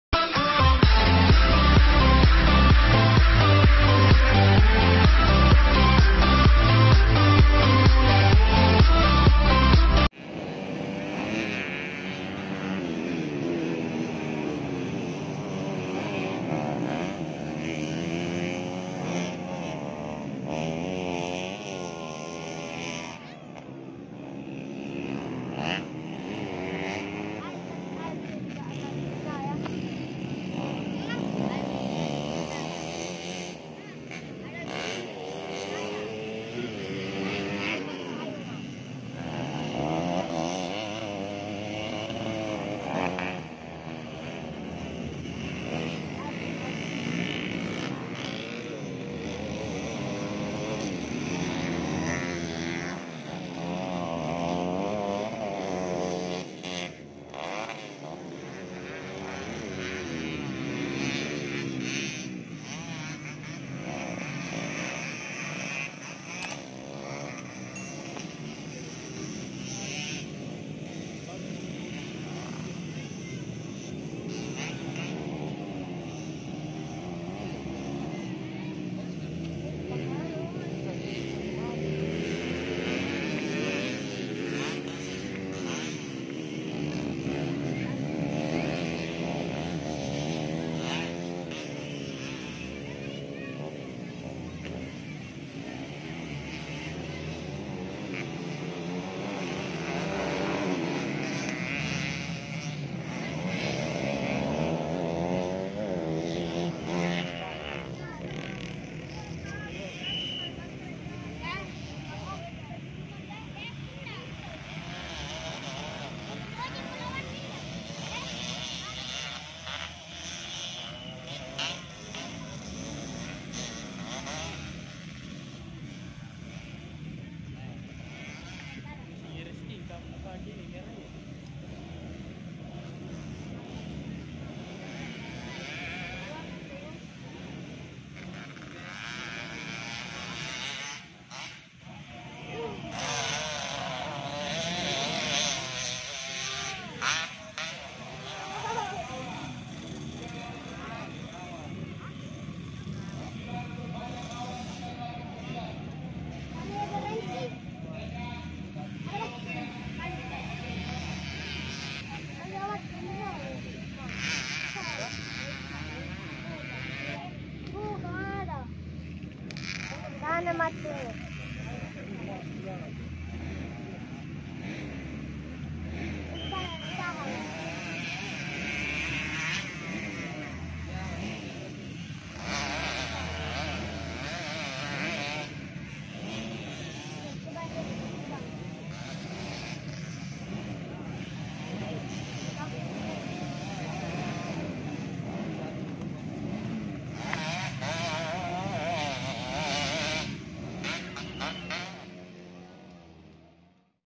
Suasana Kejuaraan Nasional Grasstrack Makassar Motocross Championship 2022
Grasstrack Regional 5 Sulawesi Putaran 2 Di Sirkuit CPI Makassar